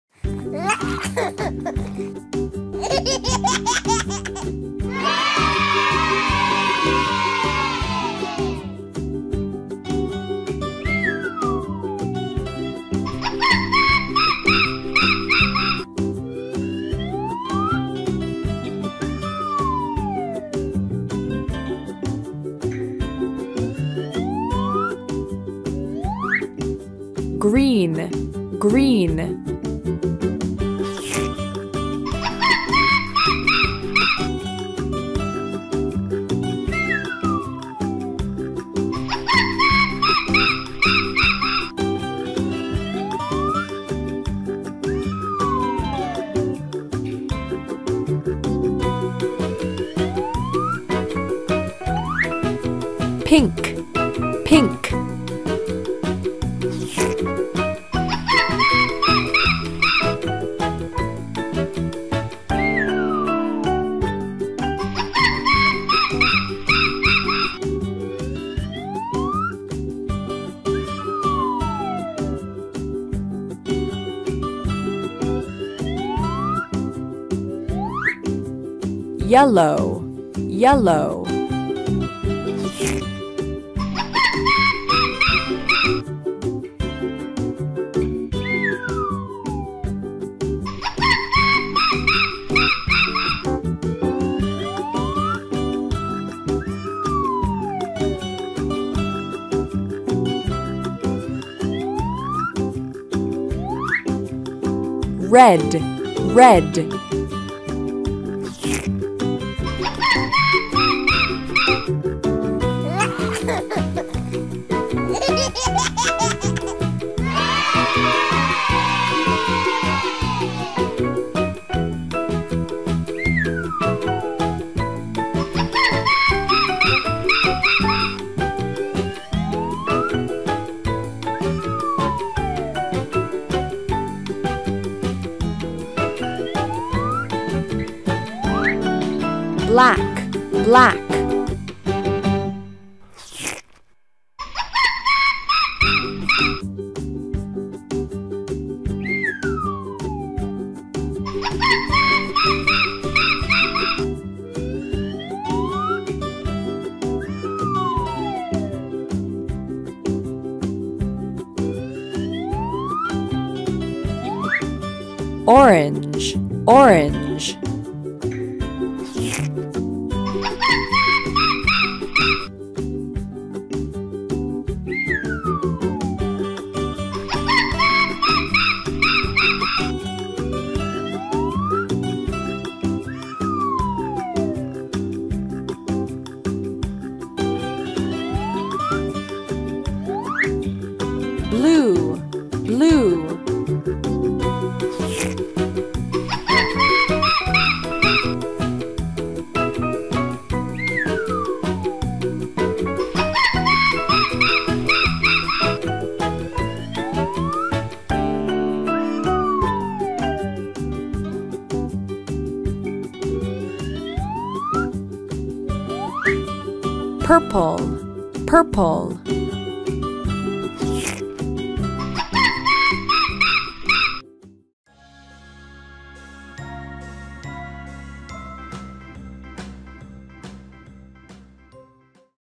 learn color yummy ice cream with monkey eating ice-cream and Nursery Rhymes
This video will helps kids learn colors from yummy ice cream. In the video, has one monkey alway want to eating ice-cream of babies. This is so funny and i hope your kids and babies love it.